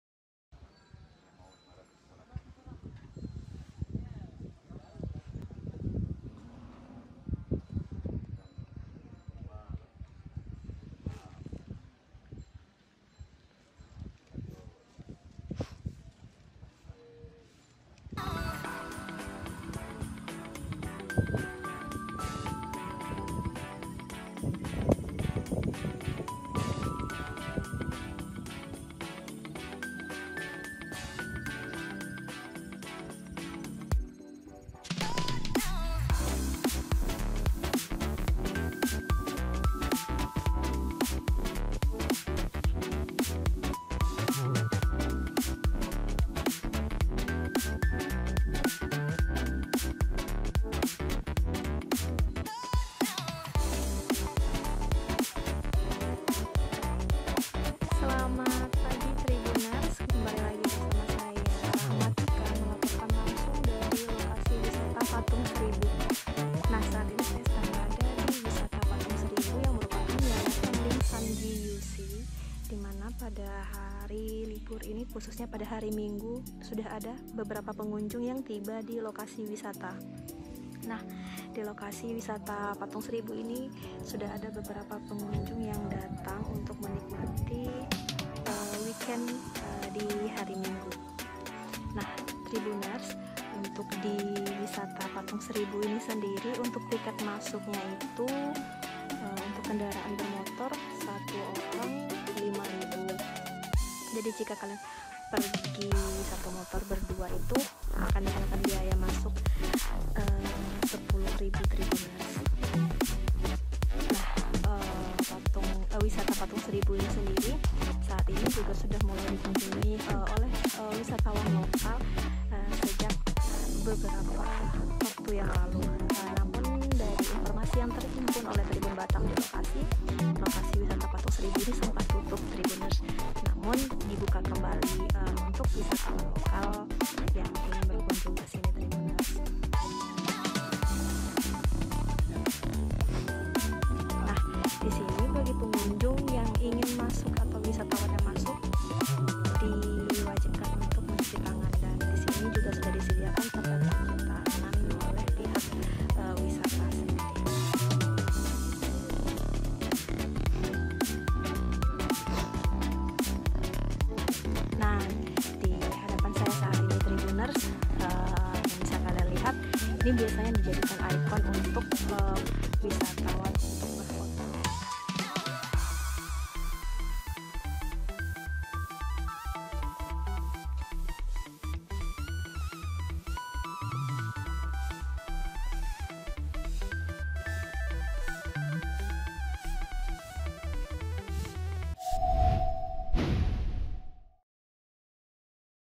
Wisata Tanjungpinang, Keunikan Wisata Patung Seribu Wajah
Keunikan Wisata Patung Seribu Wajah

Laporan Rahma Tika

Wisata patung 1000 Tanjungpinang jadi pilihan kunjungan wisatawan lokal pada hari Minggu (20/2/2022)

editing W